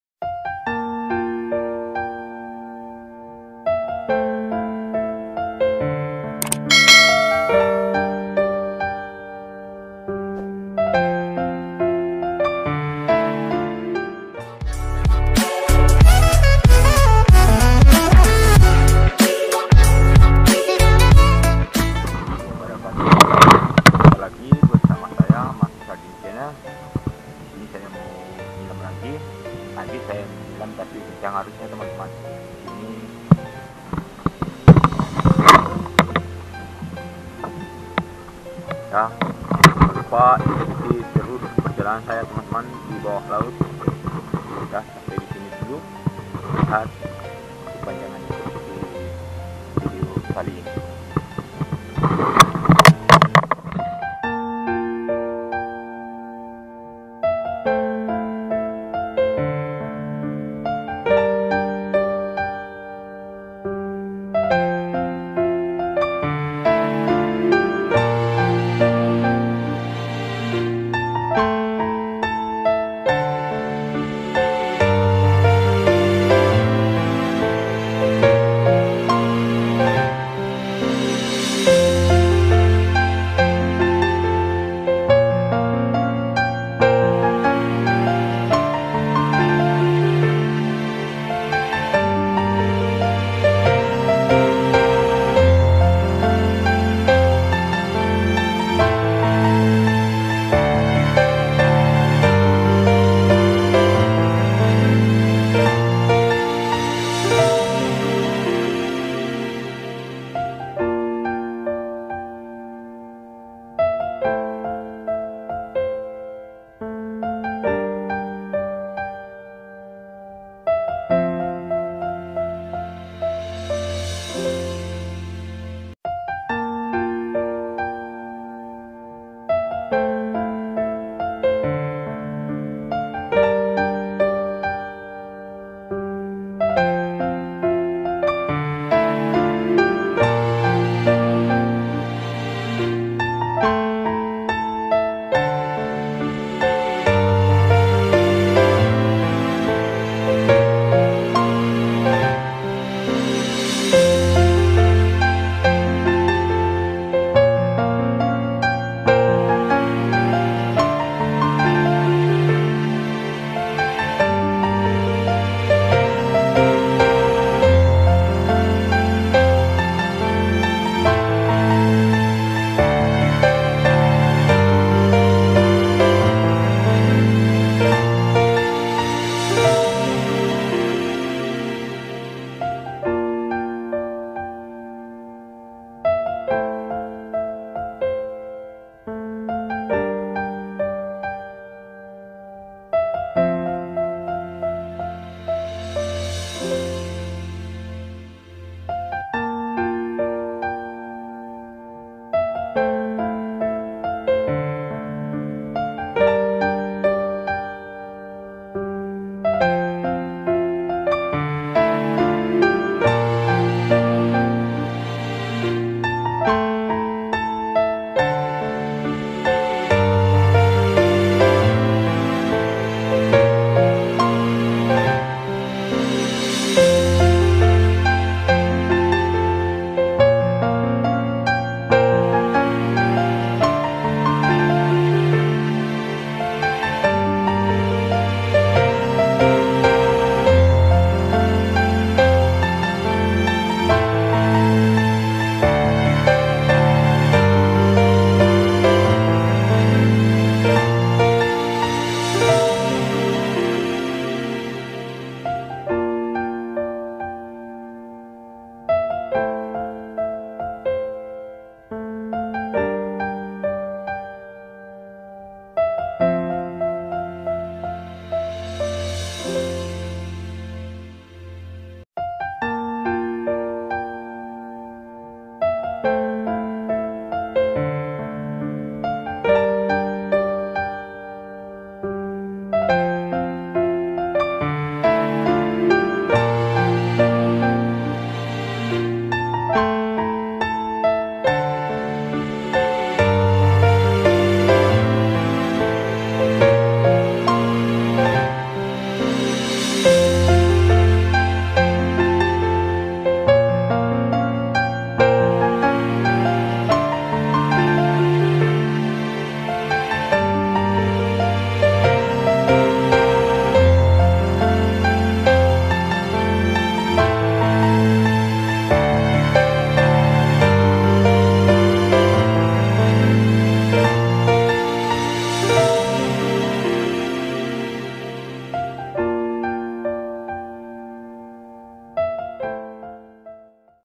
Di sini Banyak Jenis Coral, Yang Belum Melihat langsung Bawah Laut di sini
disinijeniscoral lengkapsamanamanya. jangan lupa di bantu YouTube Masradin Channel tentang bawah laut